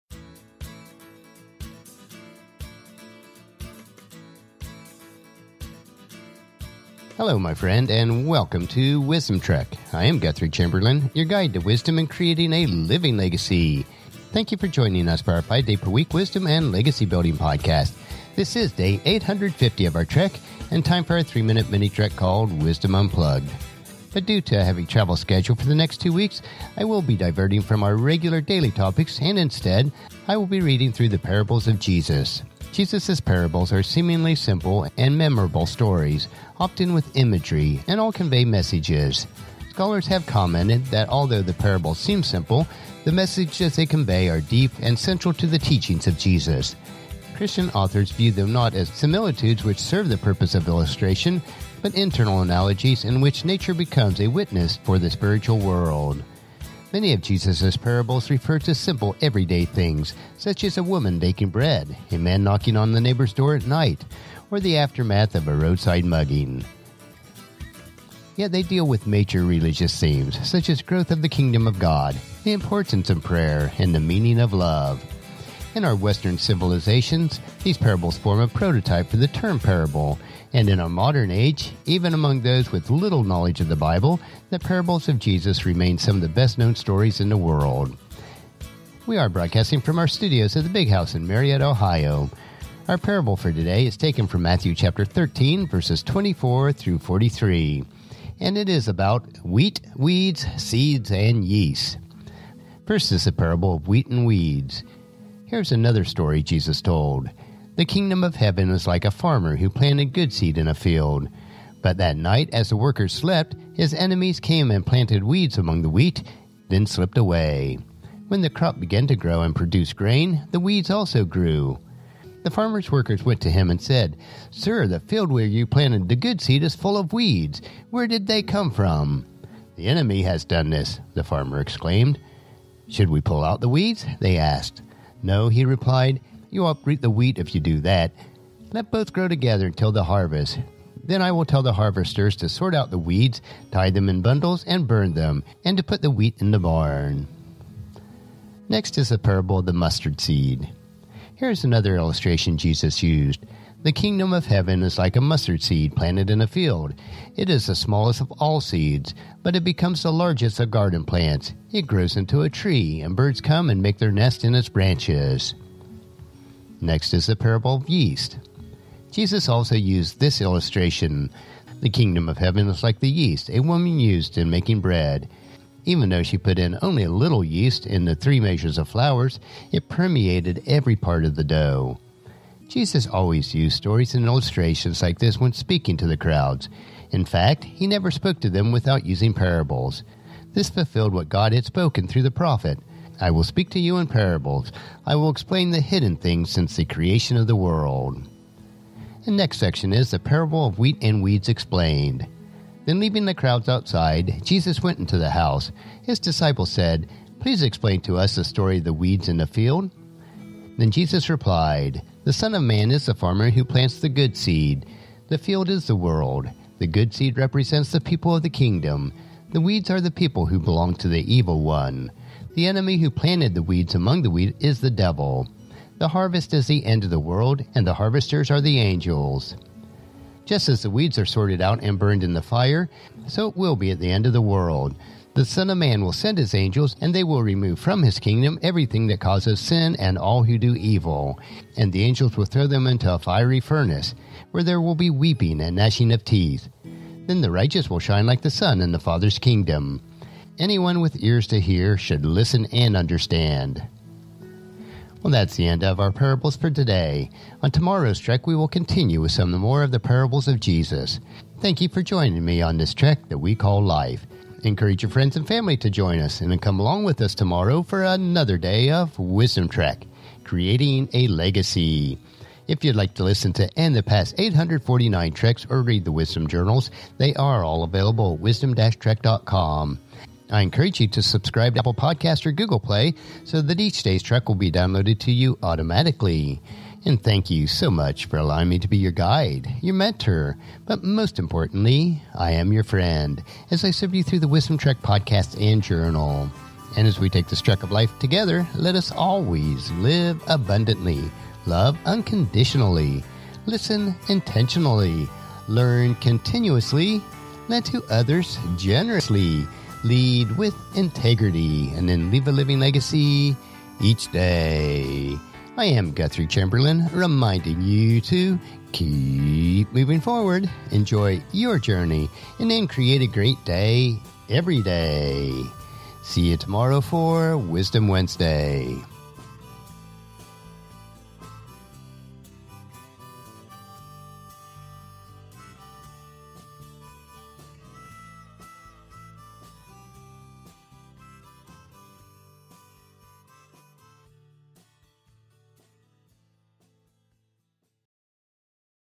Day 850 – Wheat, Weeds, Seeds and Yeast – Wisdom Unplugged
Wheat, Weeds, Seeds, and Yeast – Wisdom Unplugged

Thank you for joining us for our five days per week wisdom and legacy building podcast. This is Day 850 of our trek, and it is time for a 3-minute mini-trek called Wisdom Unplugged. Due to a heavy travel schedule for the next two weeks, I will be diverting from our regular daily topics, and instead, I will be reading through the Parables of Jesus.

Jesus's parables are seemingly simple and memorable stories, often with imagery, that all convey messages. Scholars have commented that although these parables seem simple, the messages they convey are deep and central to the teachings of Jesus. Christian authors view them not as mere similitudes, which serve the purpose of illustration, but as internal analogies in which nature becomes a witness for the spiritual world.

Many of Jesus's parables refer to simple everyday things, such as a woman baking bread. A man knocking on his neighbor's door at night. Or the aftermath of a roadside mugging. Yet they deal with major religious themes, such as the growth of the Kingdom of God, the importance of prayer, and the meaning of love.

In Western civilization, these parables formed the prototype for the term parable, and in the modern age, even among those who know little of the Bible, the parables of Jesus remain some of the best-known stories in the world.

We are broadcasting from our studio at The Big House in Marietta, Ohio. Our parables for today are taken from Matthew 13:24-43 and are about...
Wheat, Weeds, Seeds, and Yeast

· Parable of the Wheat and Weeds
Here is another story Jesus told: The Kingdom of Heaven is like a farmer who planted good seed in his field. But that night as the workers slept, his enemy came and planted weeds among the wheat, then slipped away. When the crop began to grow and produce grain, the weeds also grew.

The farmer’s workers went to him and said, "Sir, the field where you planted that good seed is full of weeds! Where did they come from?"

"An enemy has done this!" the farmer exclaimed.

"Should we pull out the weeds?" they asked.

"No," he replied, "you’ll uproot the wheat if you do. Let both grow together until the harvest. Then I will tell the harvesters to sort out the weeds, tie them into bundles, and burn them, and to put the wheat in the barn."
· Parable of the Mustard Seed
Here is another illustration Jesus used: “The Kingdom of Heaven is like a mustard seed planted in a field. It is the smallest of all seeds, but it becomes the largest of garden plants; it grows into a tree, and birds come and make nests in its branches.”
· Parable of the Yeast
Jesus also used this illustration: “The Kingdom of Heaven is like the yeast a woman used in making bread. Even though she put only a little yeast in three measures of flour, it permeated every part of the dough.”

Jesus always used stories and illustrations like these when speaking to the crowds. In fact, he never spoke to them without using such parables. This fulfilled what God had spoken through the prophet:

“I will speak to you in parables. I will explain things hidden since the creation of the world.”
· Parable of the Wheat and Weeds Explained
Then, leaving the crowds outside, Jesus went into the house. His disciples said, “Please explain to us the story of the weeds in the field.”

Jesus replied, "The Son of Man is the farmer who plants the good seed. The field is the world, and the good seed represents the people of the Kingdom. The weeds are the people who belong to the evil one. The enemy who planted the weeds among the wheat is ...